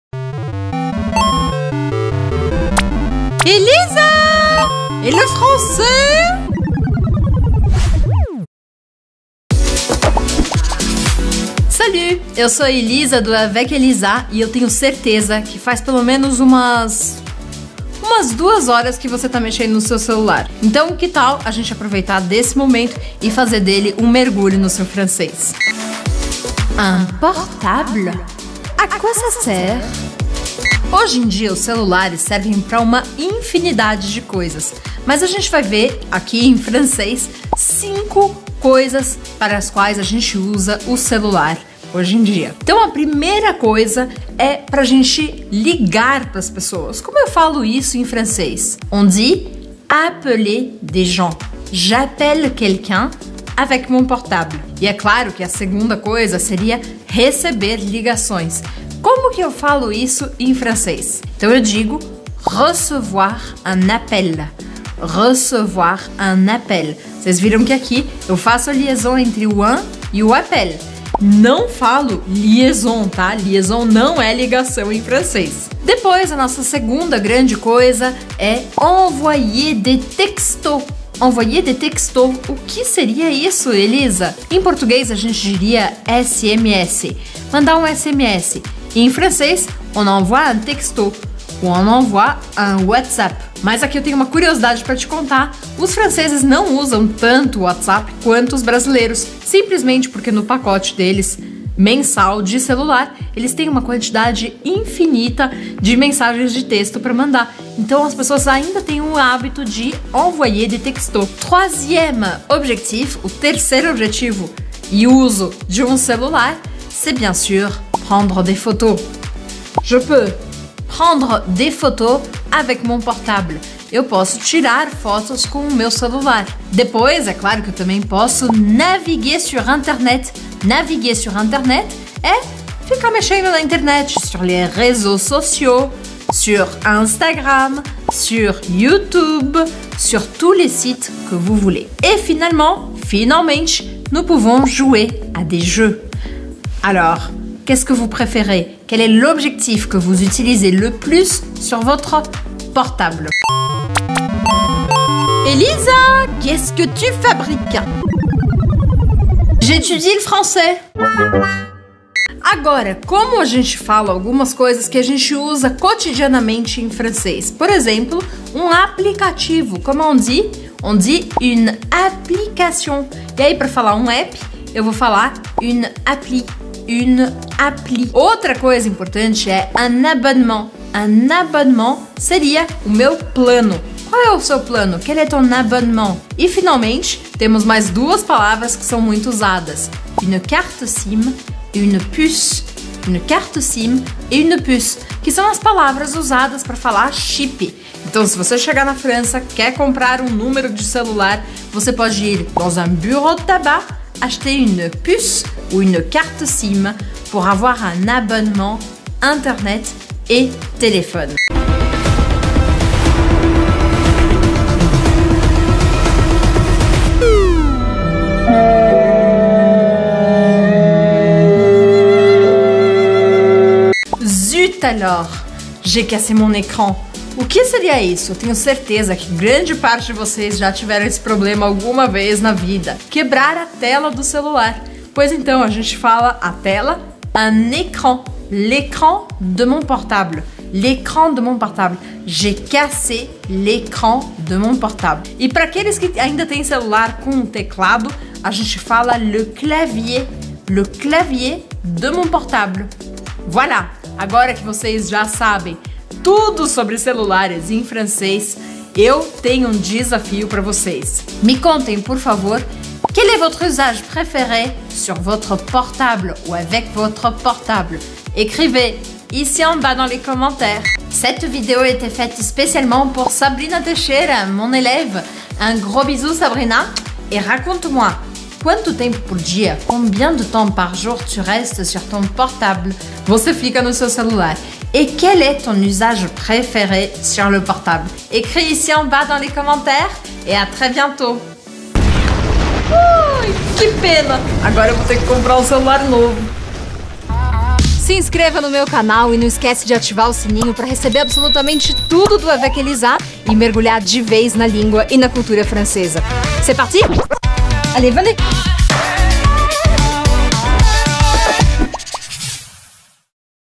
[5 Funções do seu Celular] "SMARTPHONE" em Francês
- Hoje em dia passamos muito tempo com nosso celular, pois ele tem uma infinidade de usos e funções. Por isso, e também para enriquecer seu vocabulário resolvi te apresentar em Francês, no vídeo de hoje, cinco funções básicas do seu Smartphone e super úteis no seu dia a dia. Depois de assistir tudo, me conte: "Quel est votre usage préféré sur votre portable?"

Spotify, Castbox, GoogleCast etc.